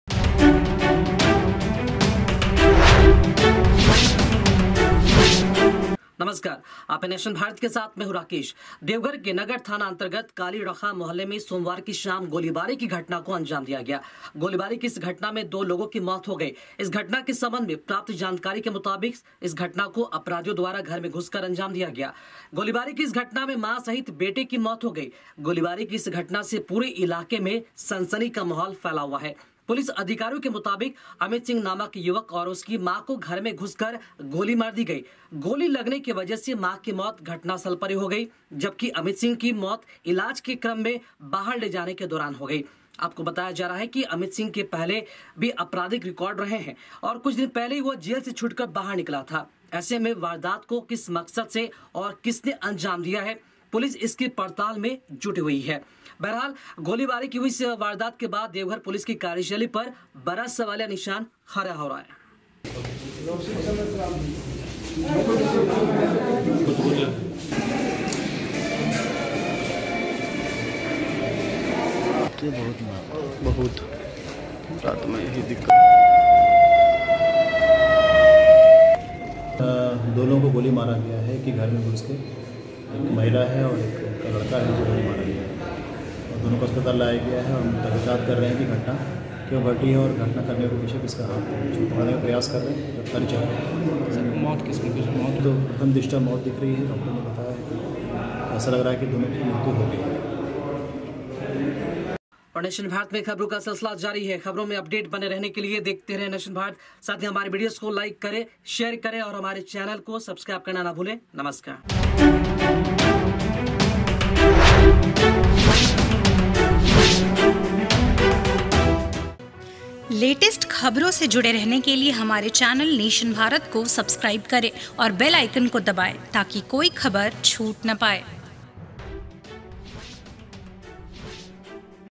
Deoghar में सरेशाम अपराधियों ने मां बेटे को गोली मार दी ,कालीराखा मोहल्ले की घटना,Police जांच में
Deoghar में  सरेशाम अपराधियों ने मां बेटे को गोली मार दी ,कालीराखा मोहल्ले की घटना
criminals shot mother and son in evening Deoghar, incident in Kalirakha locality
About Channel: